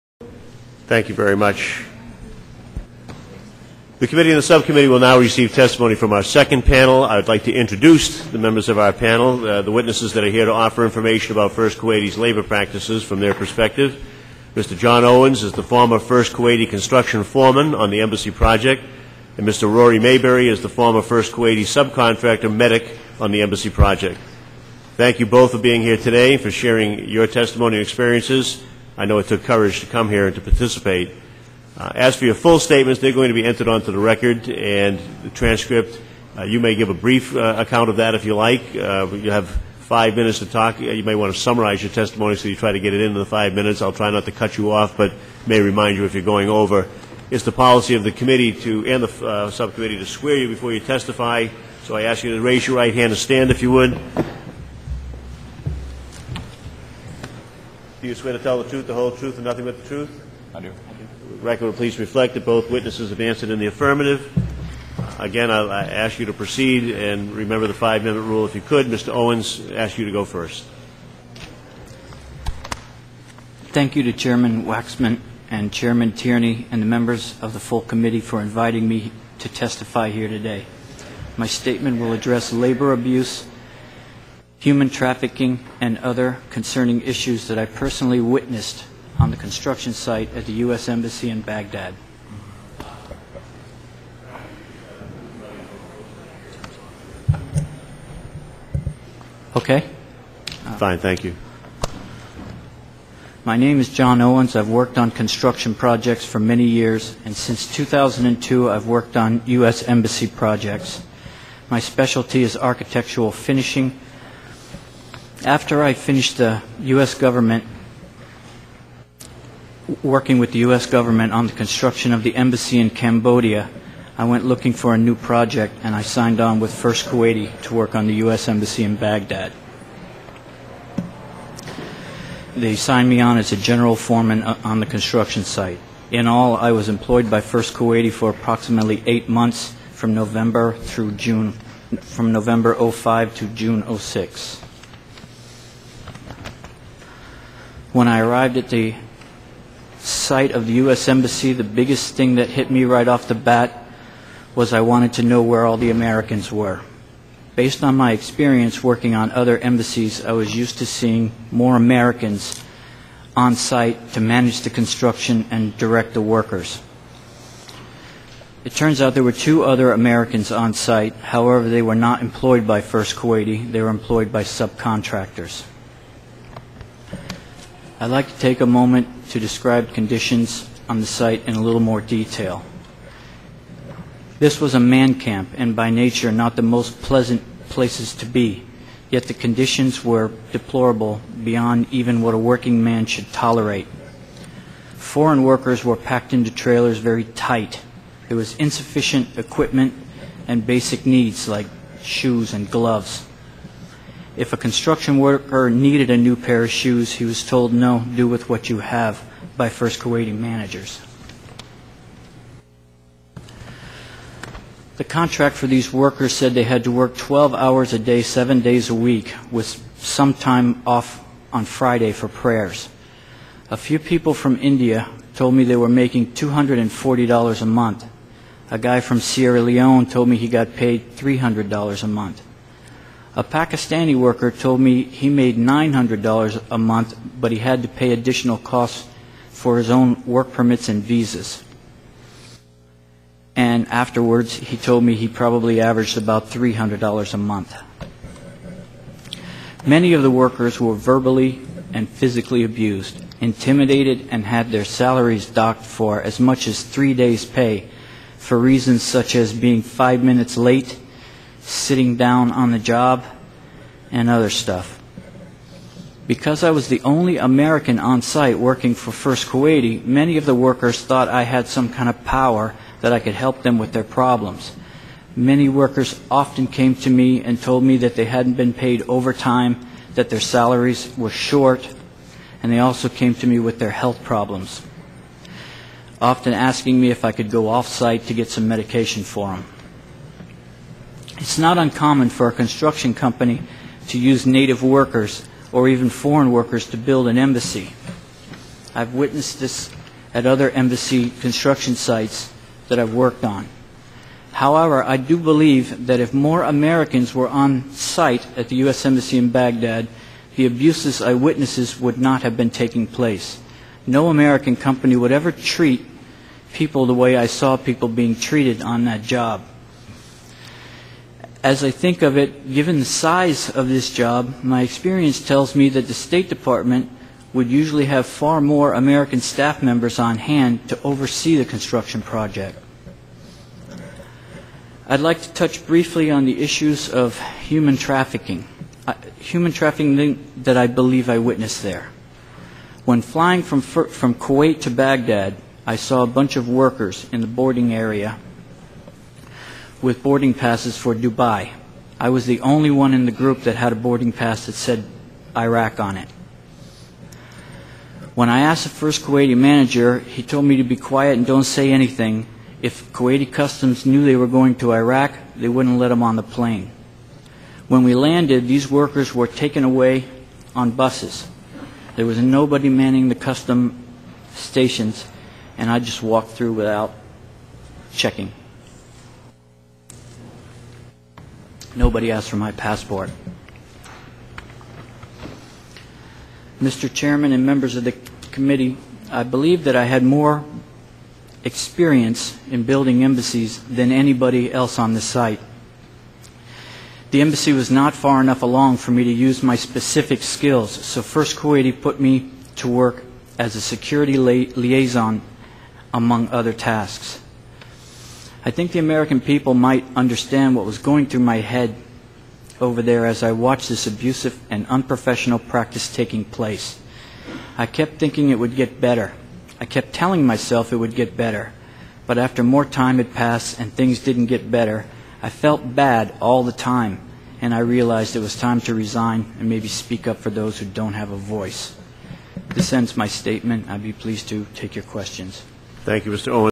Hearing on US Embassy in Iraq: Owens' Opening
The Oversight Committee holds a hearing, "Allegations of Waste, Fraud, and Abuse at the New U.S. Embassy in Iraq." The hearing examines the performance of the State Department and its contractors in the construction of the new $600 million U.S. embassy in Baghdad. The Committee reviews questions regarding the embassy compound construction as well as allegations of labor abuse through improper contracting practices.  John Owens, a former employee of First Kuwaiti Trading & Contracting Company , gives opening testimony.